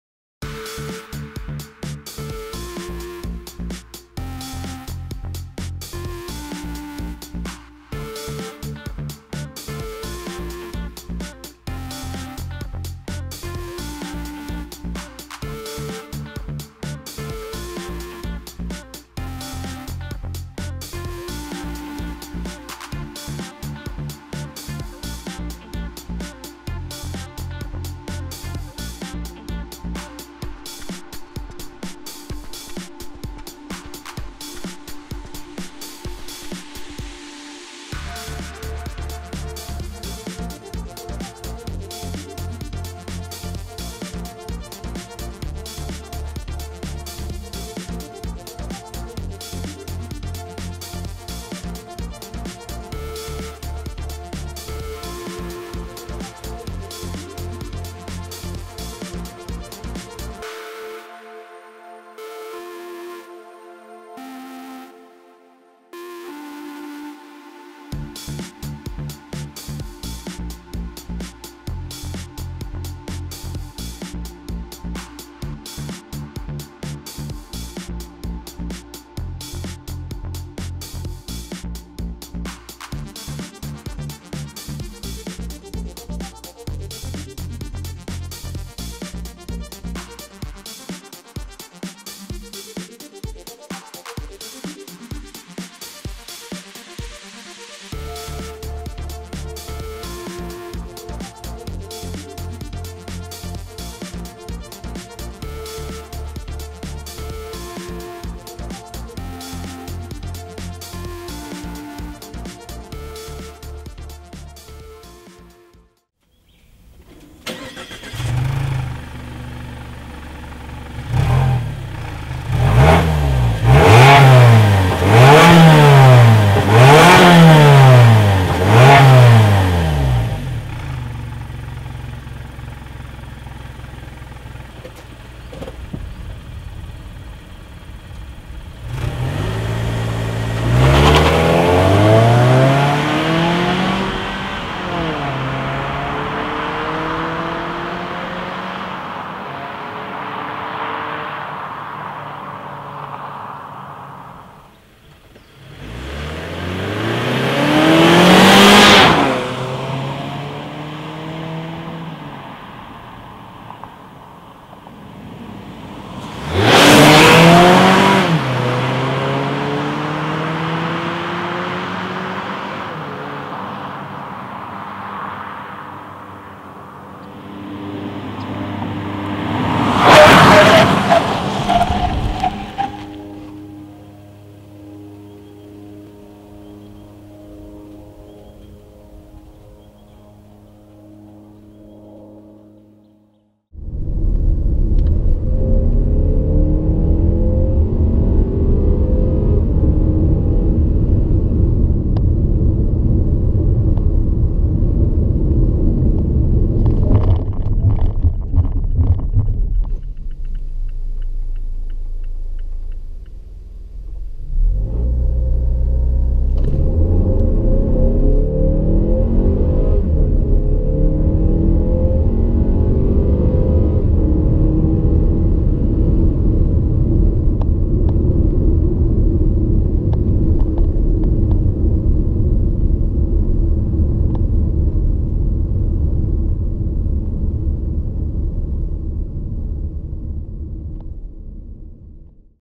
2016 Mazda6 Sport 2.5L 0-100km/h & engine sound
2016 Mazda6 Sport 0-100km/h & engine sound. for the full review.

2016 Mazda6 Sport
2.5-litre four-cylinder
138kW and 250Nm
Six-speed auto, front-wheel drive
0-100km/h in 8.03 seconds (as tested)
1/4 mile in 15.94 seconds (as tested)

For more stats and test results simply follow the link at the top of this page.